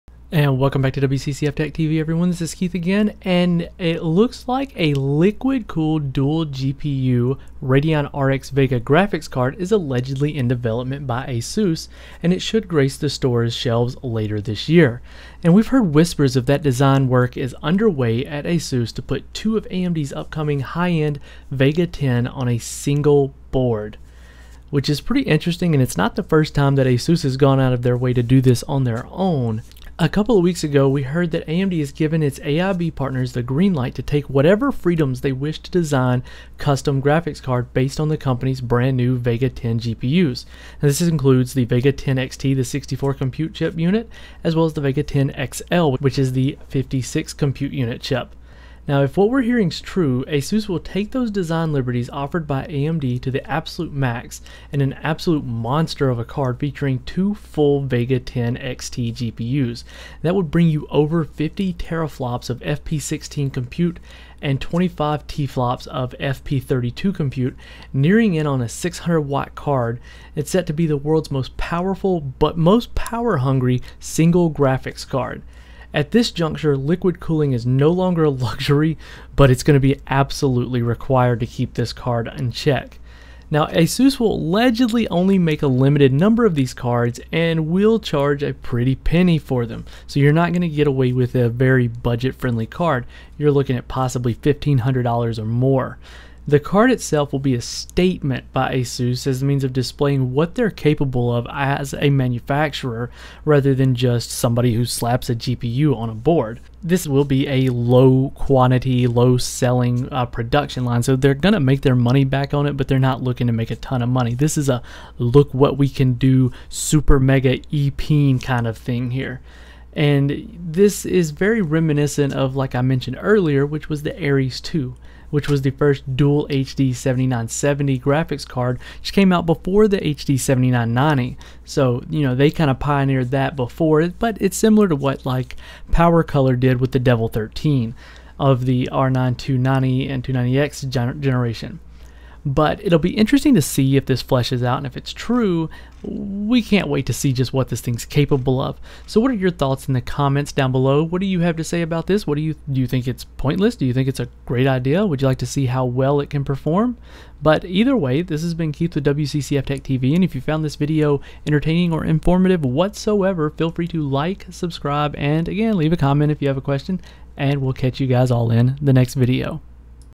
AMD RX Vega x2 Allegedly In The Works By Asus
A liquid cooled dual GPU Radeon RX Vega graphics card is allegedly in development by Asus and should grace store shelves later this year.